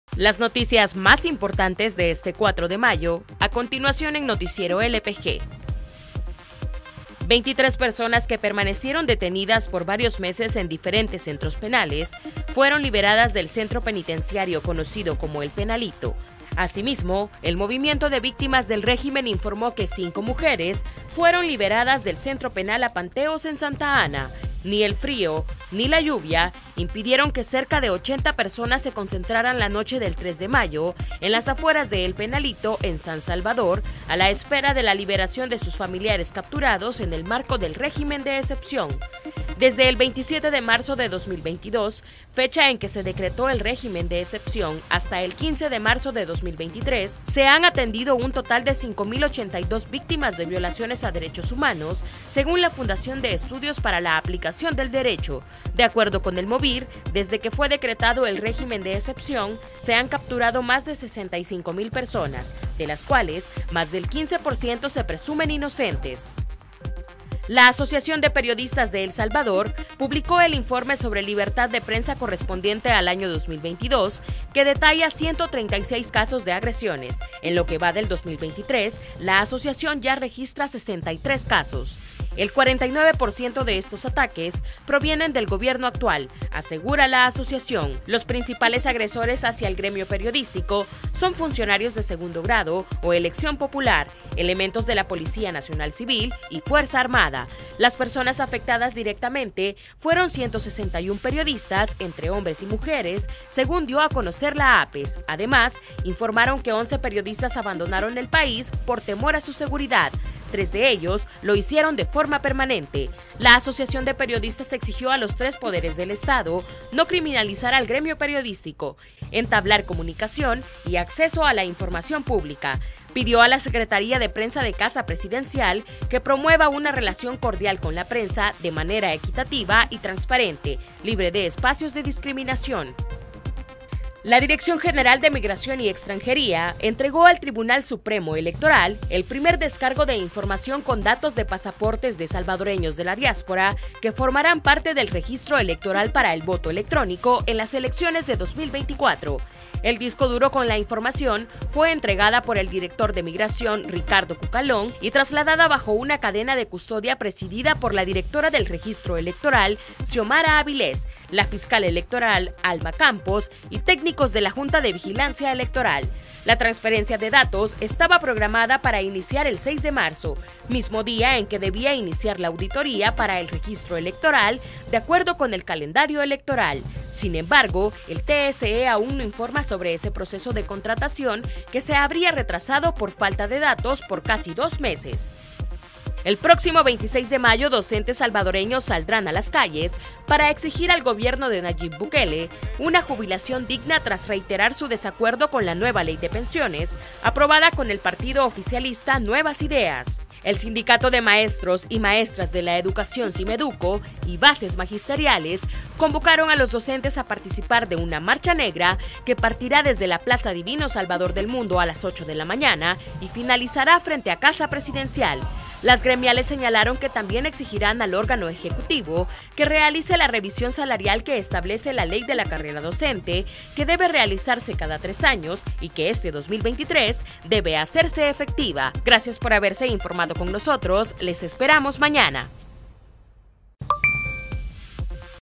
Noticiero LPG 04 de mayo: MOVIR reporta 23 personas liberadas de "El Penalito"
Ni el frío ni la lluvia impidieron que  cerca de 80 personas se concentraran  la noche del 3 de mayo en las afueras de "El Penalito" en San Salvador, a la espera de la liberación de sus familiares capturados en el marco del régimen de excepción.